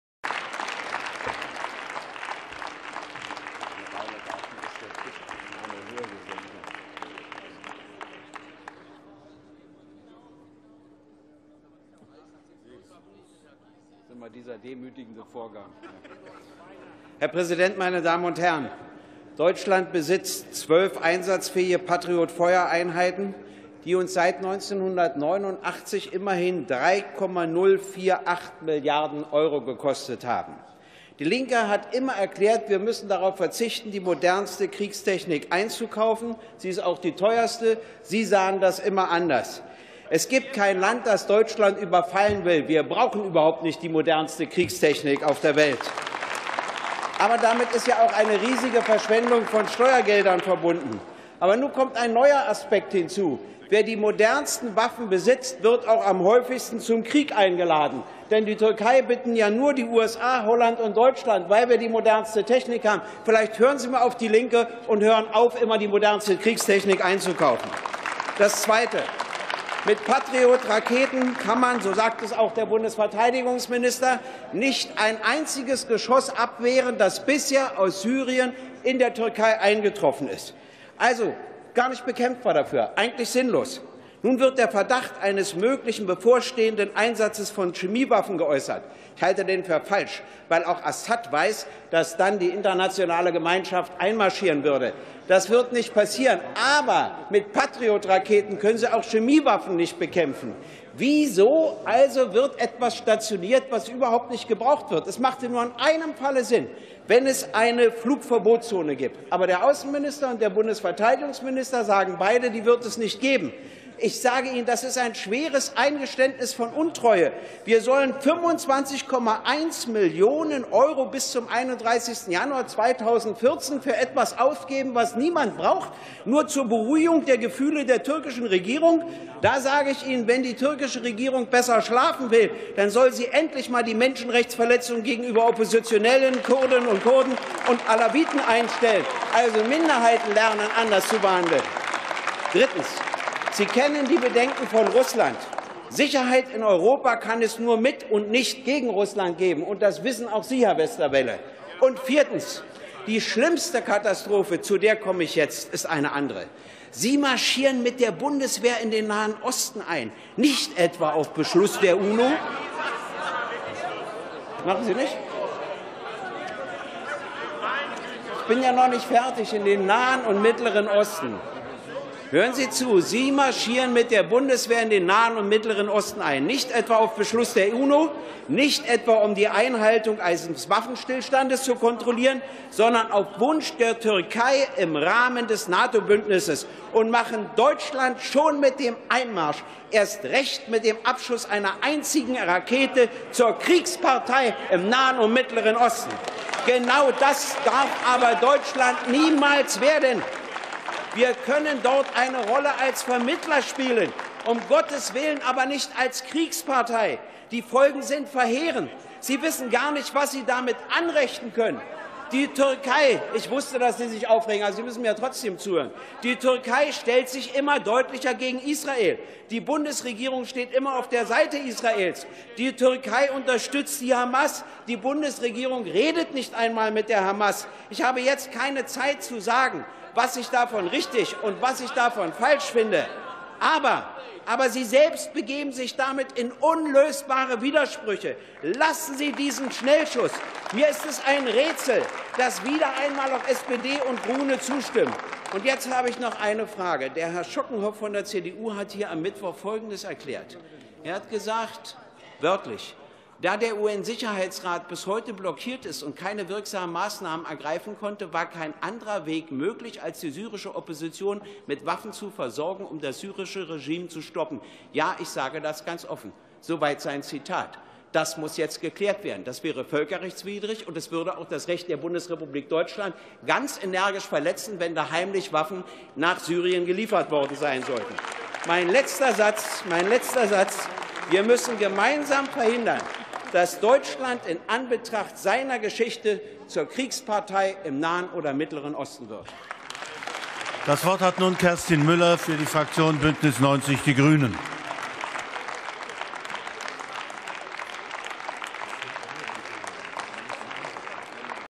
Gregor Gysi (Die Linke) -Deutschland darf sich nicht in den Krieg leiten lassen - 14.12.12
Gregor Gysi kritisiert die Kriegspolitik aller im Bundestag vertretenden Parteien.